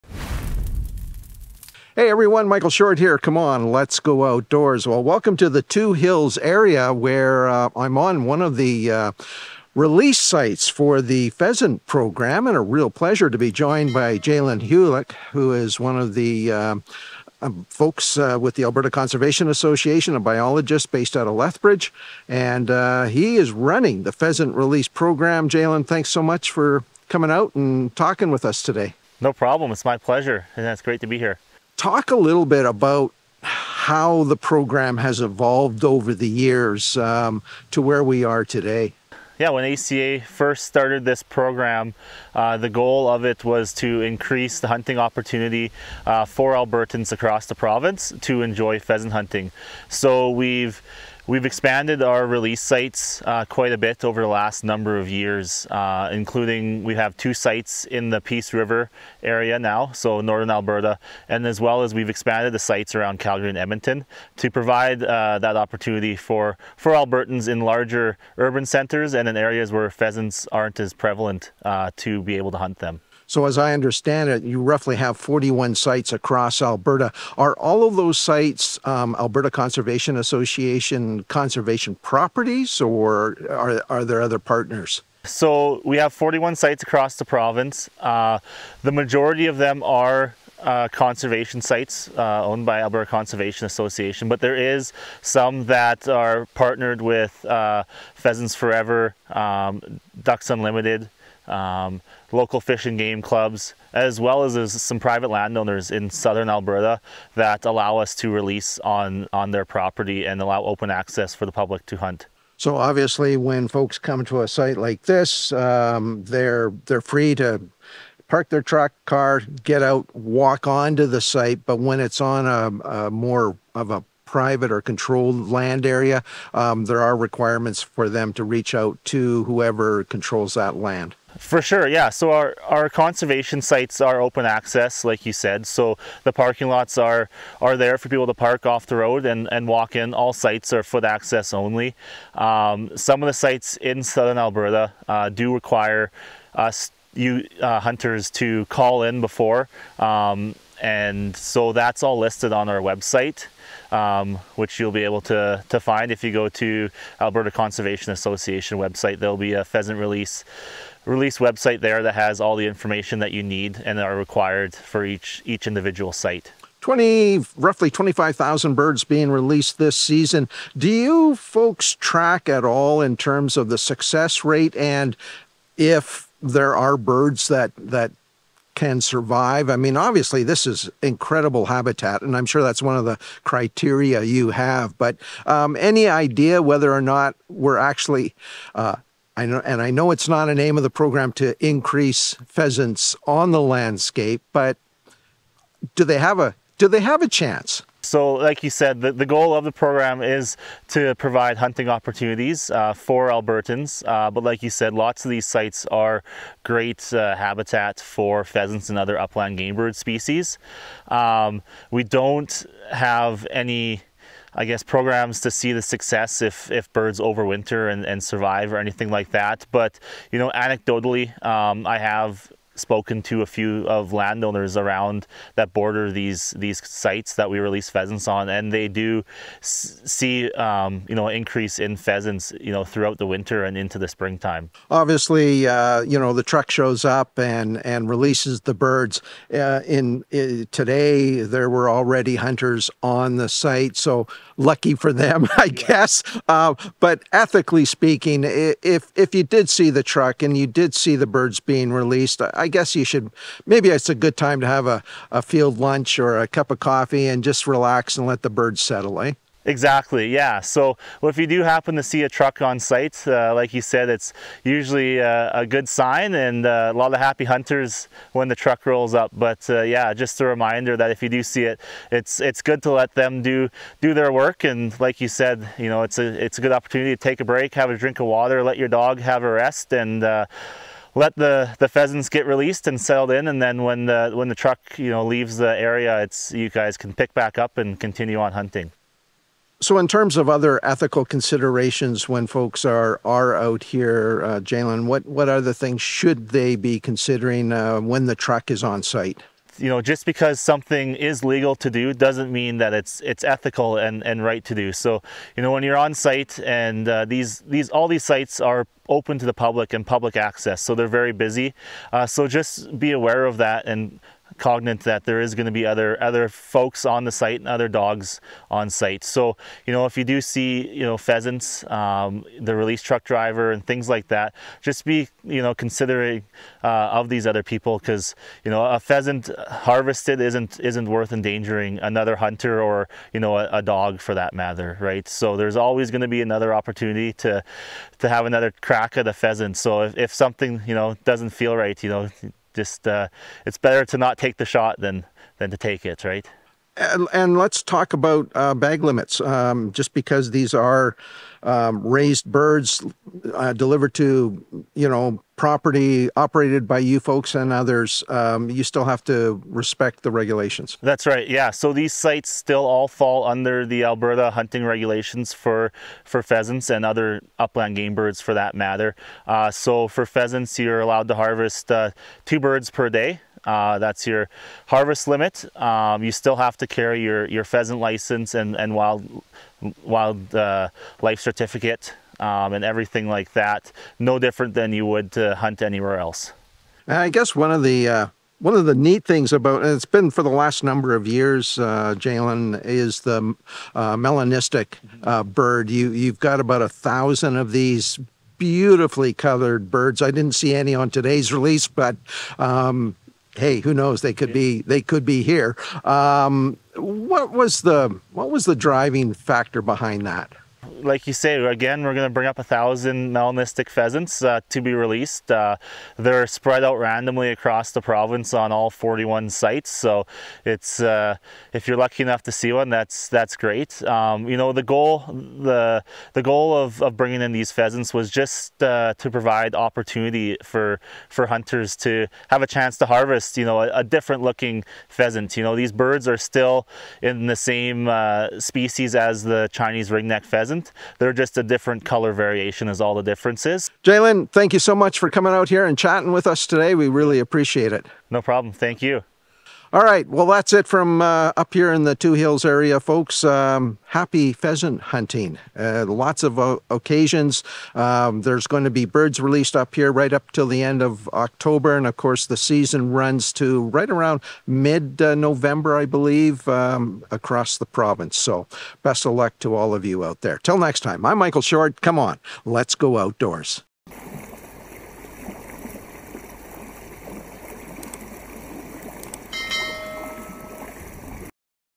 25,000 pheasants to be released in Alberta this season.
The Alberta Conservation Association runs a Pheasant release program that offers hunting opportunities at over 40 sites in Alberta. The program aims to create hunting opportunities in areas with suitable pheasant habitat. I interviewed Jalen Hulit, a biologist with the ACA and the coordinator of the Pheasant Release program, to learn more about it.